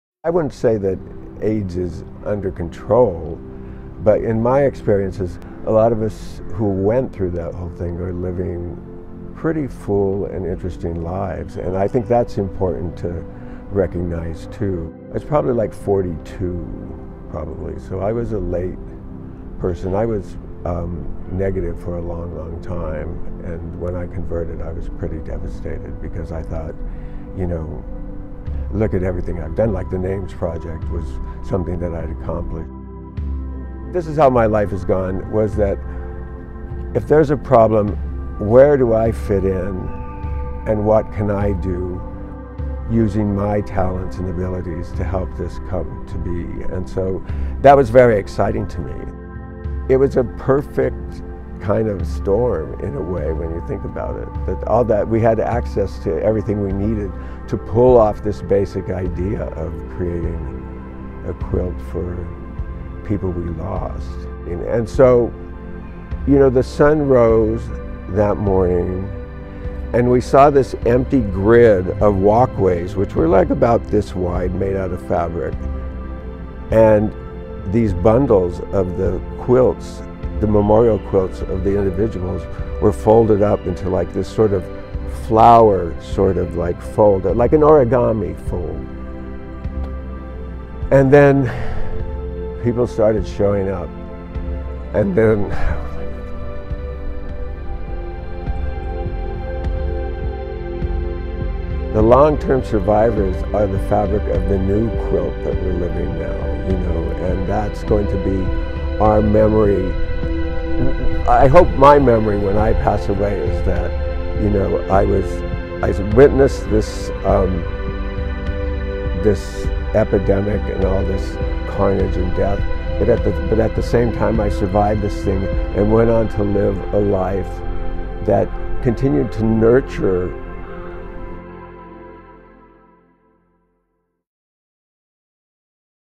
World AIDS Day 2020
Openhouse Community leader and resident, Ron Cordova, remembers another fall day in 1987 when a team of activists and artists took the AIDS Memorial Quilt for its first unfolding on the National Mall in Washington, D.C.  Ron was Technical Director for the NAMES Project/AIDS Memorial Quilt.  Today, he sits on the Openhouse Steering Committee for its Living Tribute to HIV Long-Term Survivors, an art installation completed this July in the new Openhouse Community Center. The centerpiece of the tribute, a two-story mural by San Francisco artist and HIV long-term survivor David Faulk, waits for its public unveiling and dedication until another pandemic passes. Segment Producer and Editor:  Jackie Meinhardt. Archival video footage courtesy of KQED. Still images courtesy of Ron Cordova.